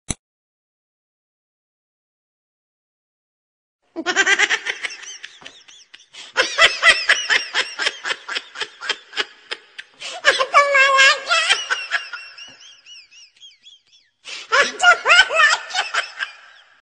Il nous fait trop rire ce petit singe
Mort de rire  ce petit singe trop mignon